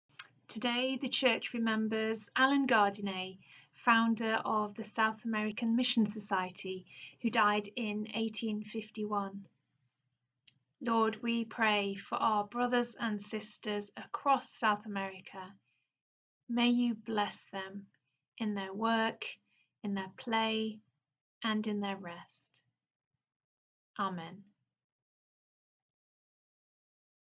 Everyday Prayer 6 September 2020
Today, Emma Nutbrown Regional Media Officer @christian_aid prays for all our brothers and sisters in South America.